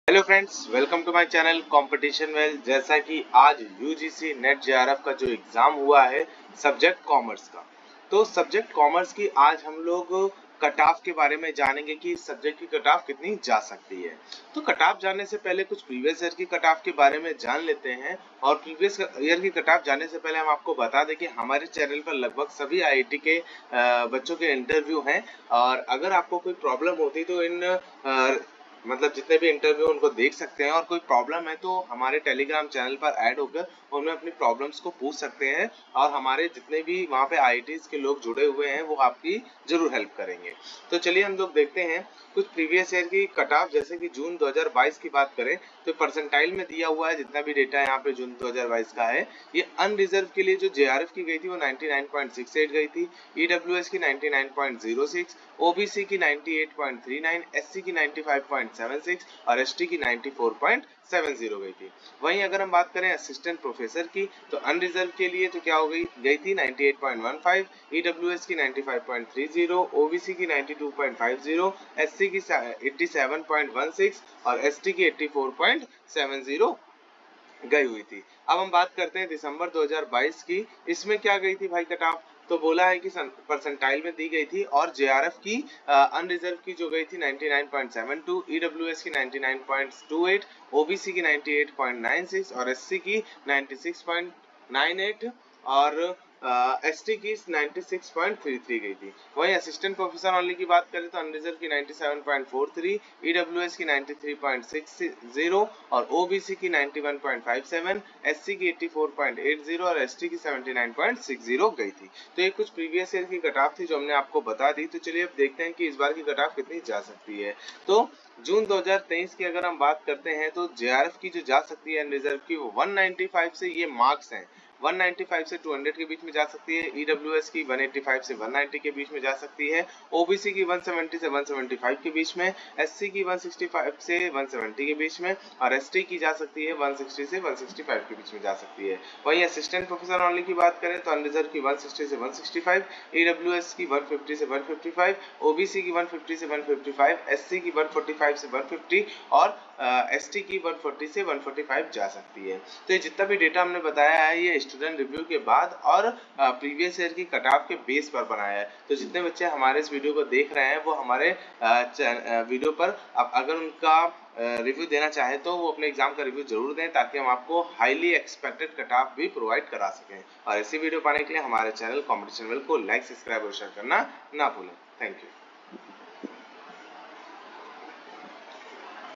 UGC NET JRF JUNE 2023 | CUT OFF MARKS of COMMERCE
In this video, we explain the expected cut off of UGC NET JRF (COMMERCE) June 2022 and also we explained Previous Years cut off of Dec. 2022, june 2020,DEC. 2019,June 2019,Dec. 2018, .
Note :- All the previous Years Cut offs data we take From NTA UGC NET official Website and Google.